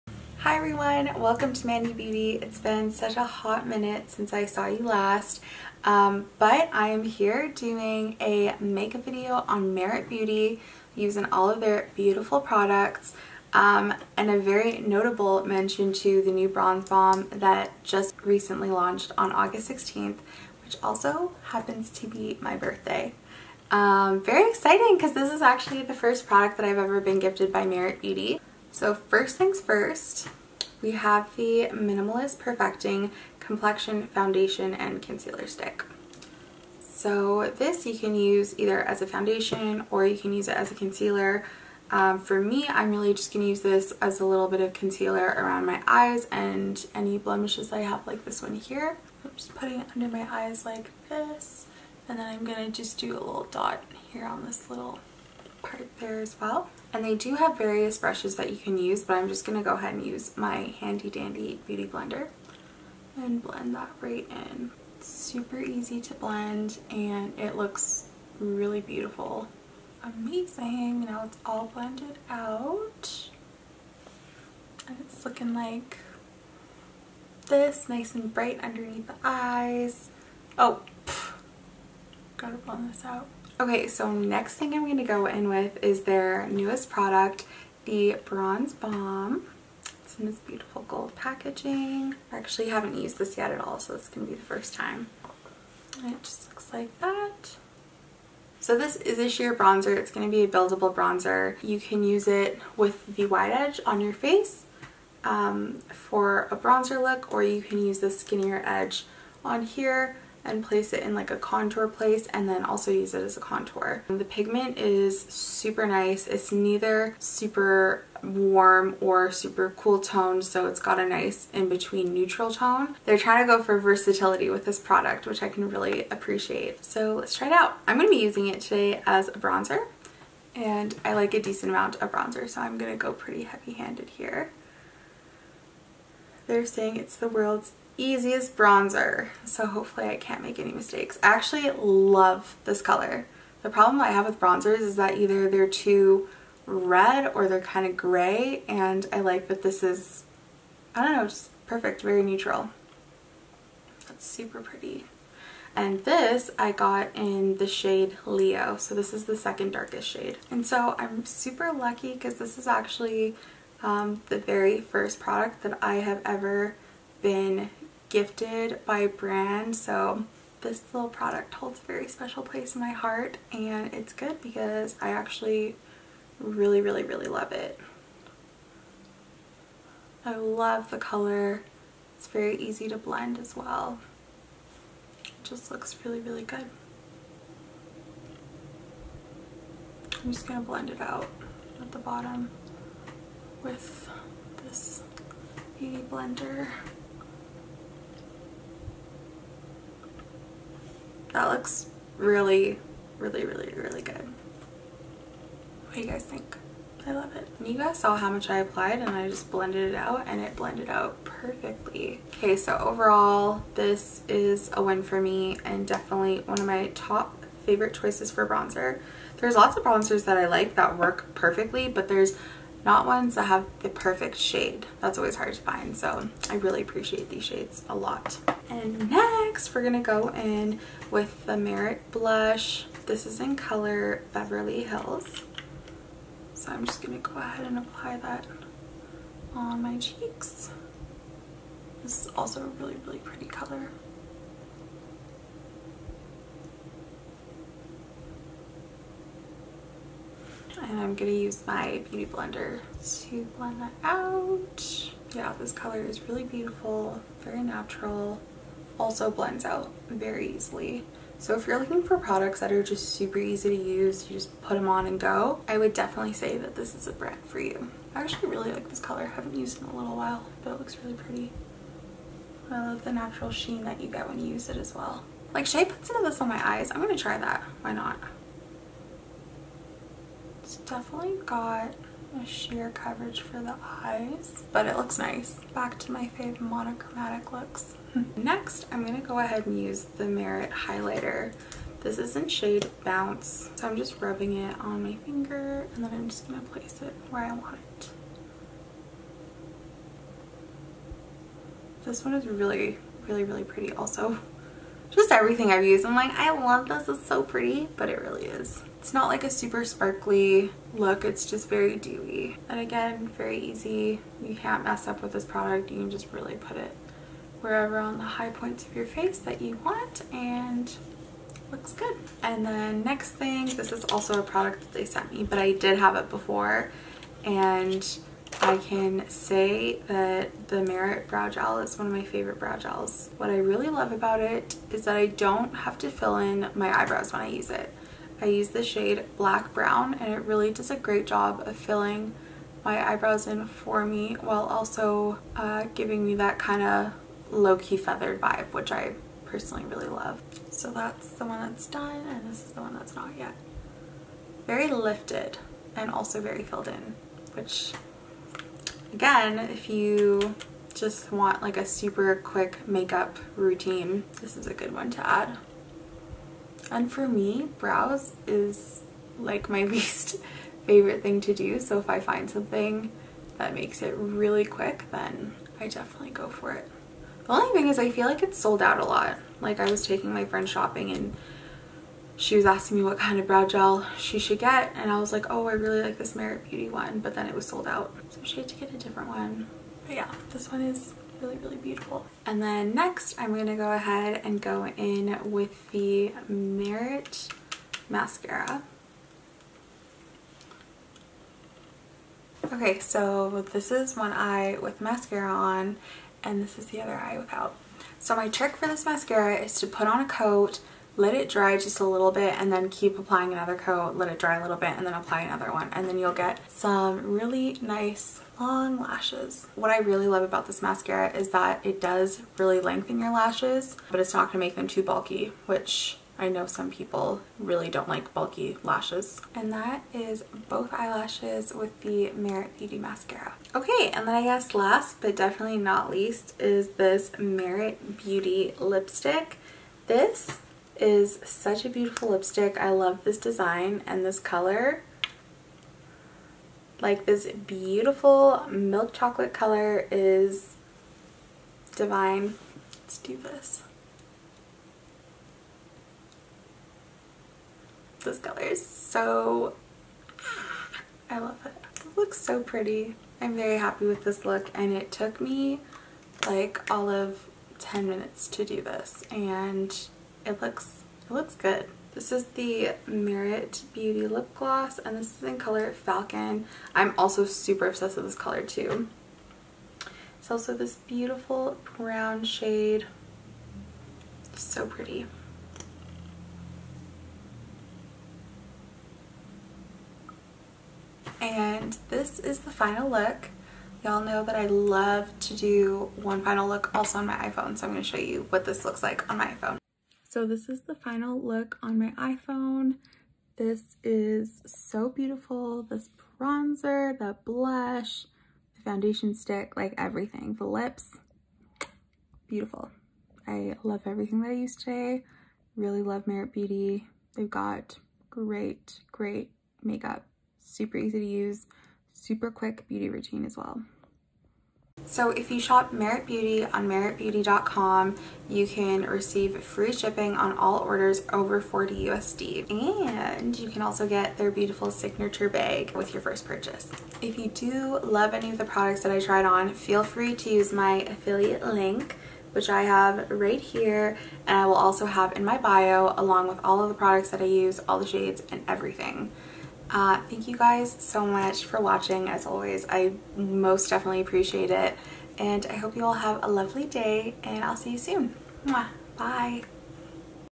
Full face of MERIT Beauty | NEW BRONZE BALM TRY ON!
(FYI, if you do use this link, I do make commission on the products you purchase!)

For my first video of 2022, I am wearing a full face of MERITBeauty! This clean at Sephora makeup brand is known for high-quality products that provide the classic clean girl aesthetic.

MERIT recently released it's Bronze Balm - a multi-use bronzer formula that is bendable and lightweight, with a satin/radiant finish. And best of all, it's got a neutral tone!

The Minimalist PERFECTING COMPLEXION STICK (Buff)
Bronze Balm SHEER BRONZER (Leo) *gifted *ADgifted
Flush Balm CHEEK COLOR (Beverly Hills)
Day Glow HIGHLIGHTING BALM (Bounce)
Brow 1980 VOLUMIZING POMADE (Soft Black) *gifted *ADgifted
Clean Lash LENGTHENING MASCARA
Signature Lip LIGHTWEIGHT LIPSTICK (Slip)
Shade Slick TINTED LIP OIL (Falcon)